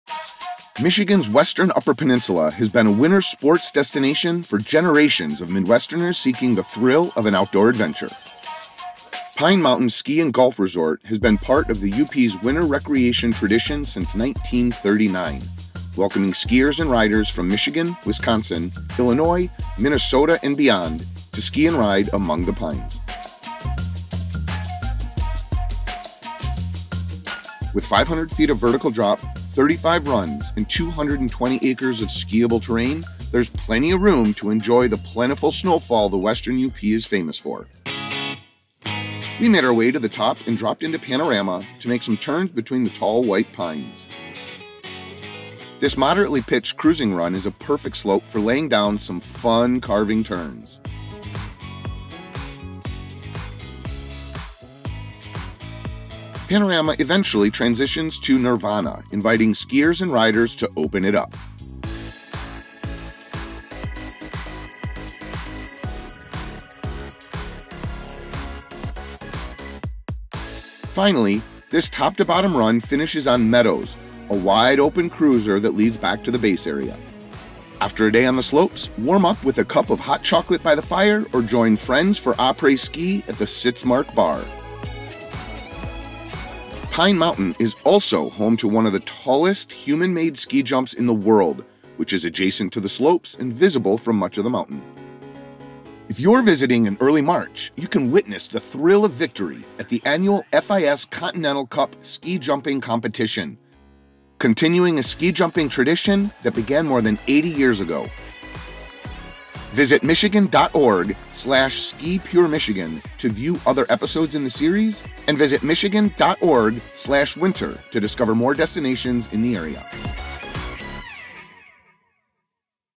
Pine Mountain | Ski Pure Michigan
Pine Mountain Ski & Golf Resort has been an upper peninsula winter tradition since 1939. With 35 runs, there is room for everyone to enjoy the snowfall.

Explore more of PureMichigan's signature ski runs